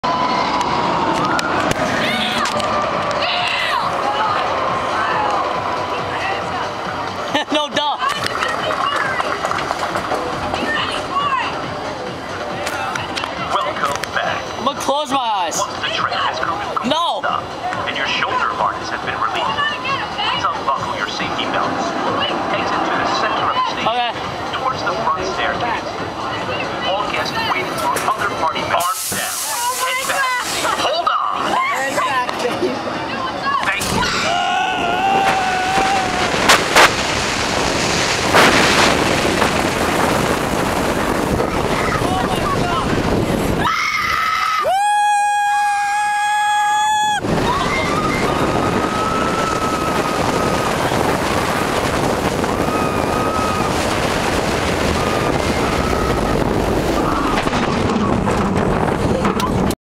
Kingda Ka HD
This is not a GREAT video, but the launch and speed REALLY created a lot of wind and feedback.

What you need to realize is that you cannot leave Six Flags Great Adventure without riding this.

It was indescribable.

Awesome.

Fast.

Just, wow.

-Jackson, New Jersey